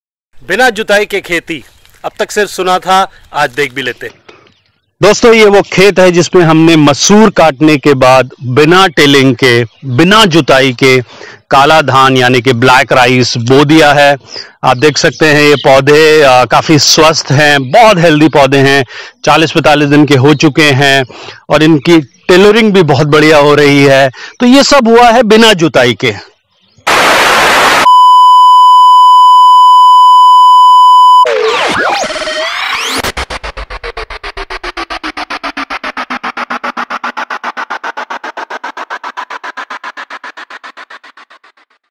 बिना जुताई की खेती | No-Till Farming | COMING SOON
We will share the pros and cons of no till farming once our pilot with black paddy is over.
While we all know how important it is to NOT disturb the soil, Its equally important to understand  the practical aspects and challenges of no tilling.
At karbanic we follow what we practice and thats exactly what we want to achieve through this pilot project.

Full video will be available for members only!